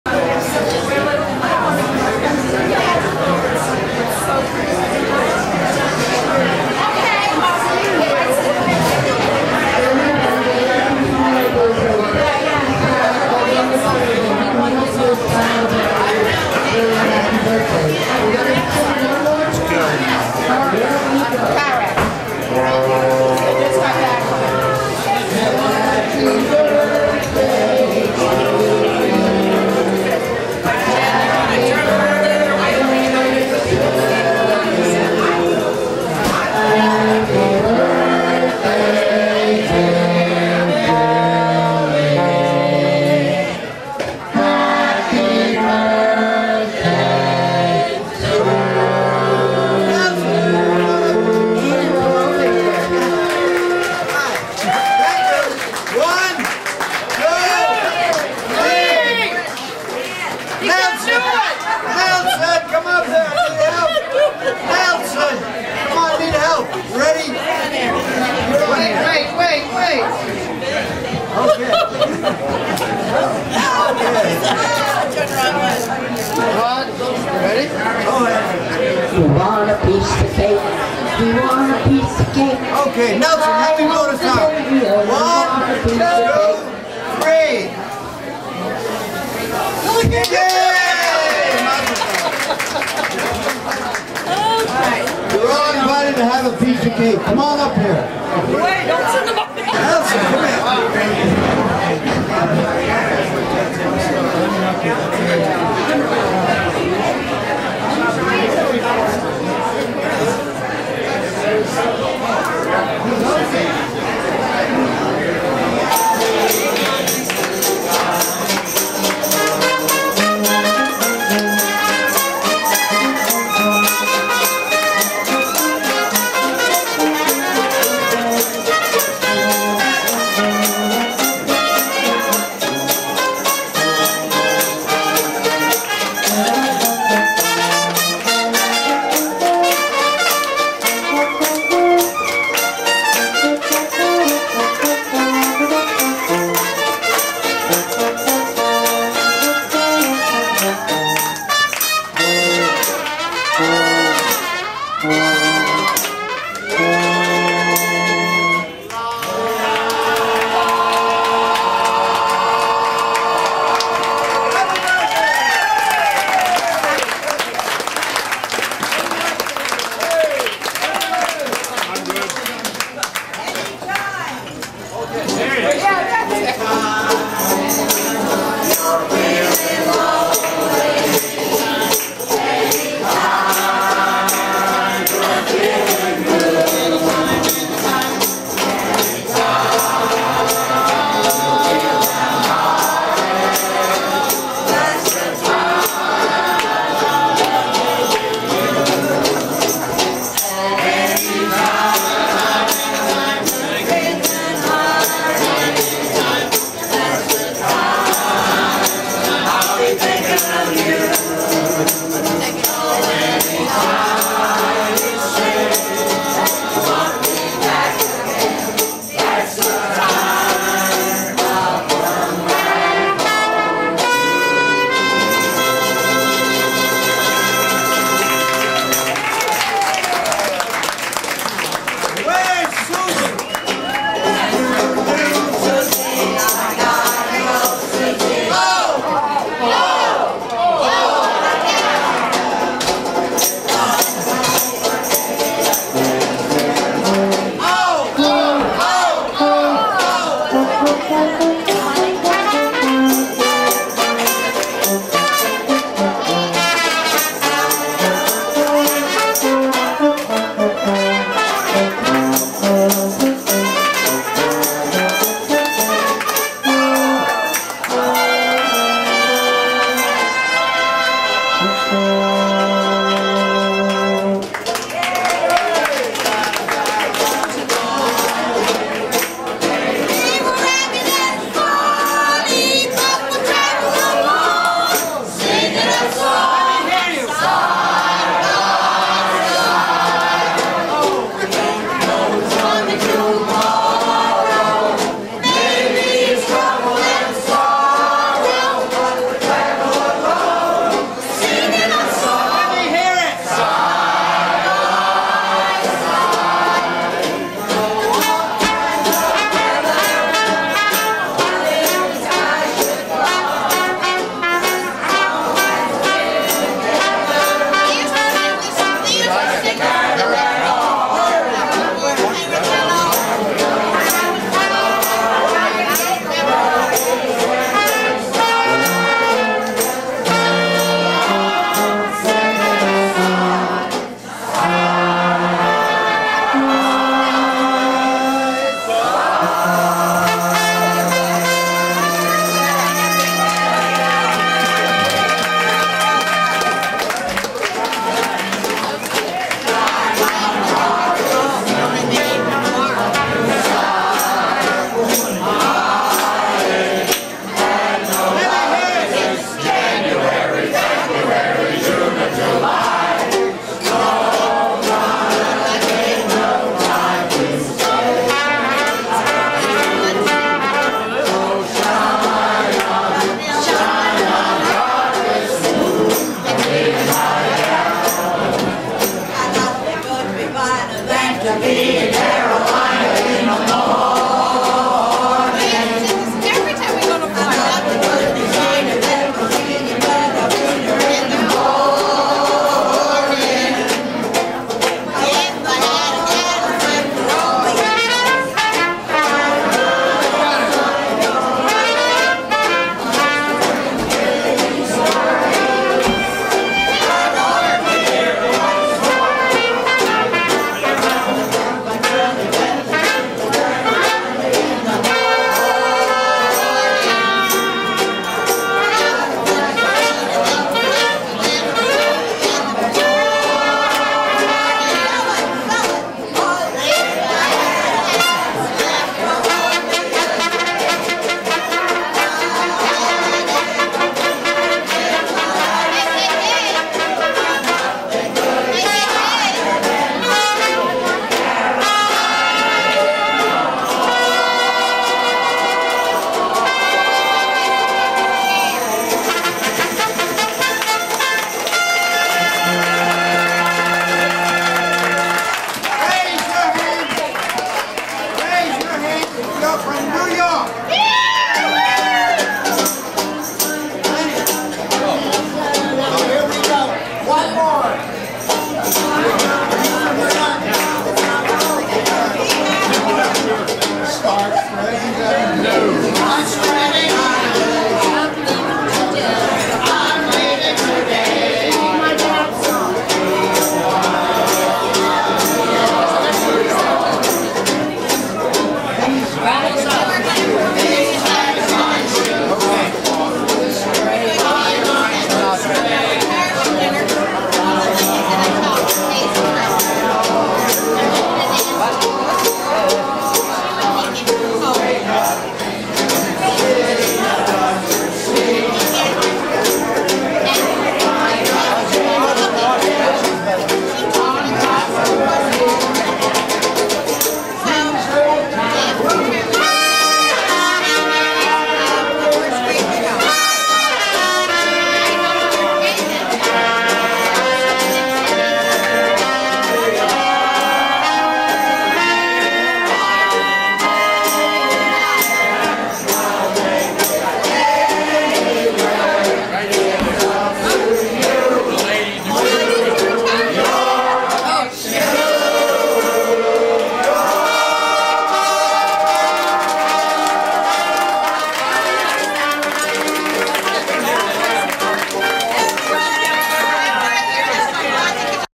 Billy Mure's 93rd Birthday Party at Squid Lips
This is a video of Billy Mure's 93rd birthday party at Squid Lips in Sebastian, Florida.